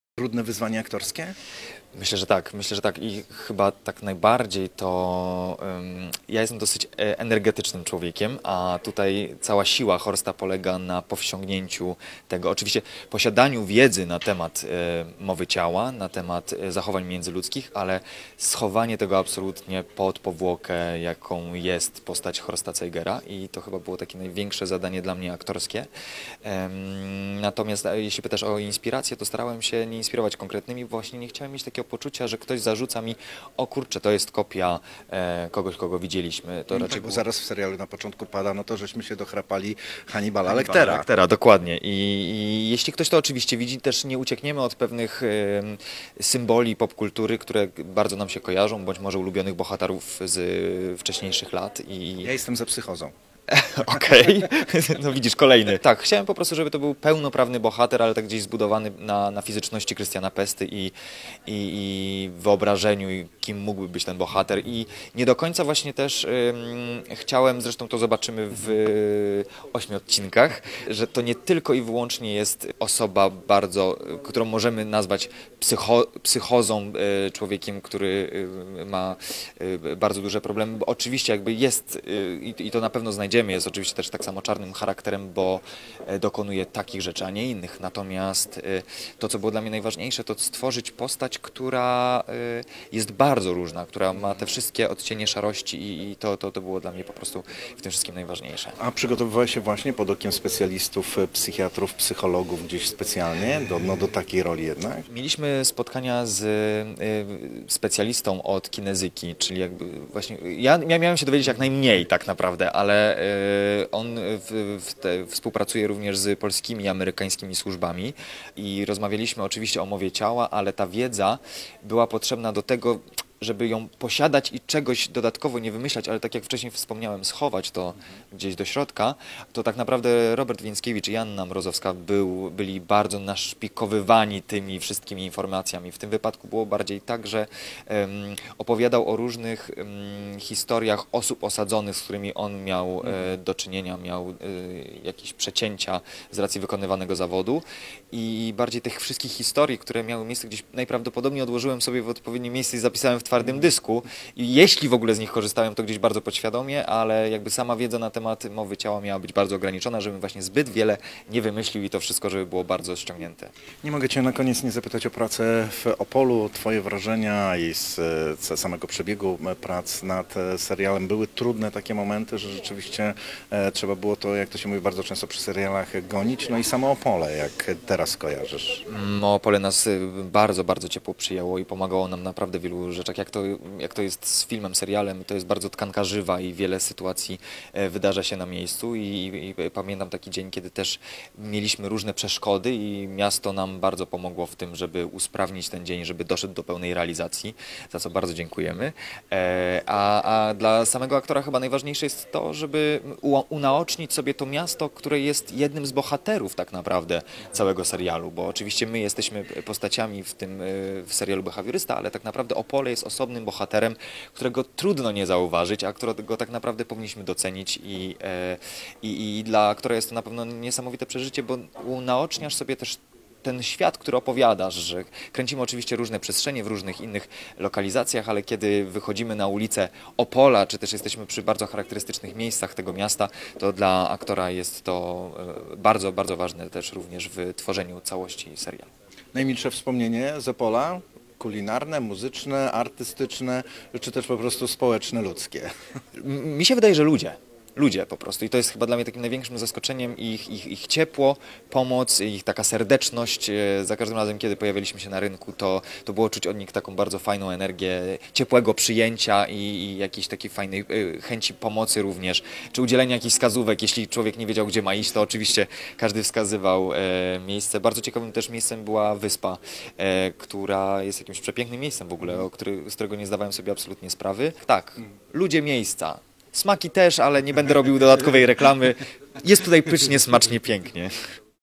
Krystian Pesta o roli w "Behawioryście"
Rzeczywiście to było wyzwanie dla mnie bo konstrukcja i rys mojego bohatera dawały z jednej strony duże pole do popisu a z drugiej wymagało ogromnych przygotowań - tak o swojej roli w serialu "Behawiorysta" mówi Krystian Pesta.